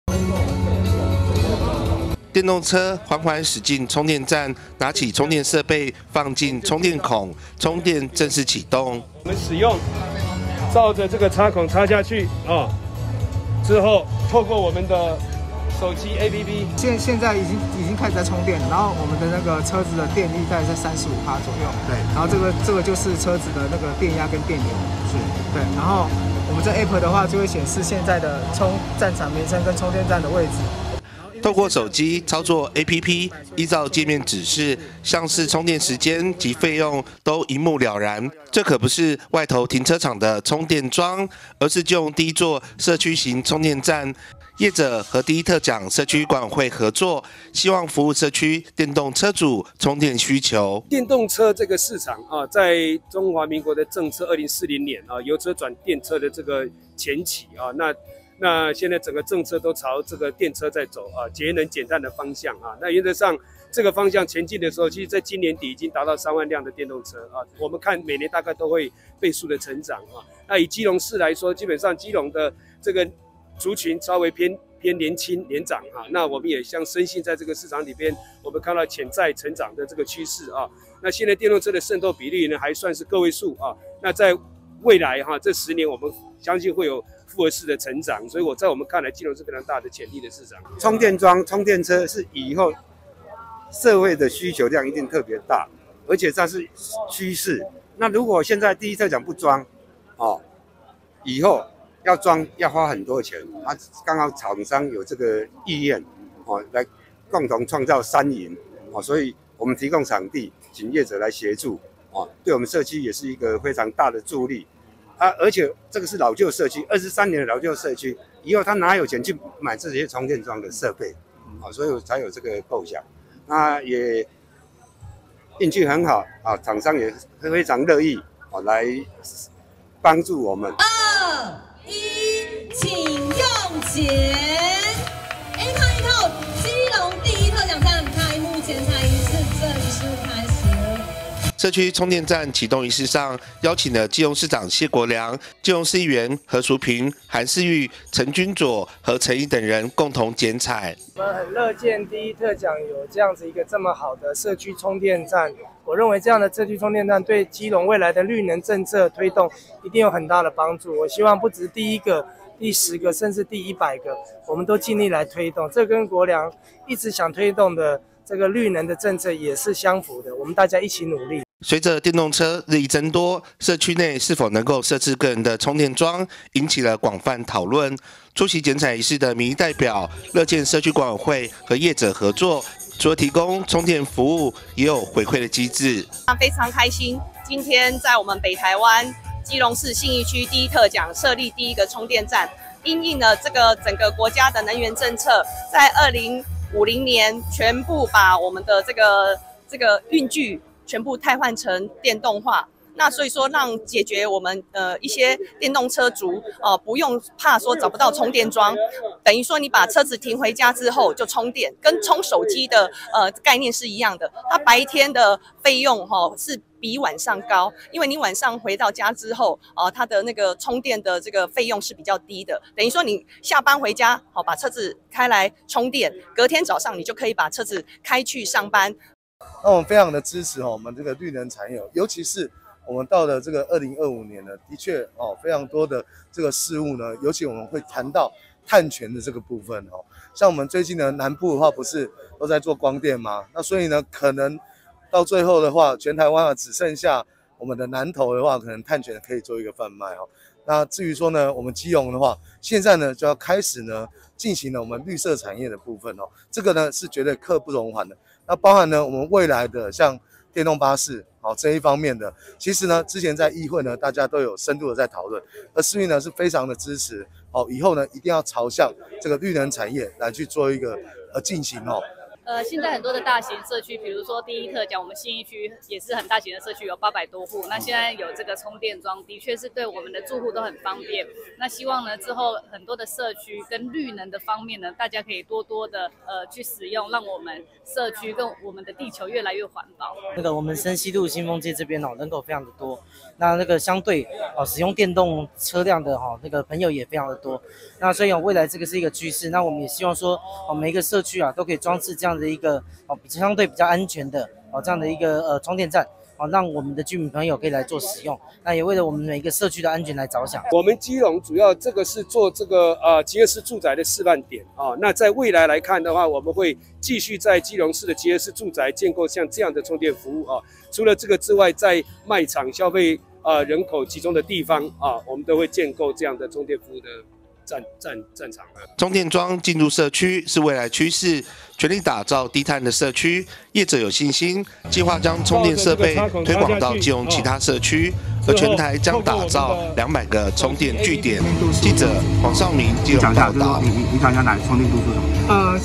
電動車｜首座快充停車位進駐社區 謝國樑主持剪綵
(中嘉新聞記者黃紹銘/基隆採訪報導) 電動車充電樁越來越普及，基隆第一特獎社區管委員與業者合作，設置基隆市第一座社區型充電樁據點，除了提供電動車主充電服務，也將所得回饋社區，基隆市長謝國樑親自主持剪綵，樂見一起打造低碳、綠色環保節能的城市。
電動車緩緩駛進充電站，拿起充電設備放進充電孔，充電正式啟動。
透過ＡＰＰ依照指示，充電時間及費用勸都一目了然，這可不是外頭停車場充電樁，
而是基隆第一座社區型充電站，業者與第一特獎社區管委員合作，希望服務社區電動車主，充電需求。
社區充電站啟用儀式上，邀請基隆市長謝國樑、基隆市議員何淑萍、韓世昱、陳軍佐、陳宜等人依同剪綵。
隨著電動車日益增多，社區內是否能設置個人充電樁，引起廣泛討論，出席剪綵儀式的民意代表，
樂見社區管委會與業者合作，除了提供充電服務，也有回饋機制。
充電樁進駐社區，是未來趨勢，全力打造低碳的社區，業者有信心，計畫將充電設備推廣到基隆其他社區，而全台將打造200充電樁據點。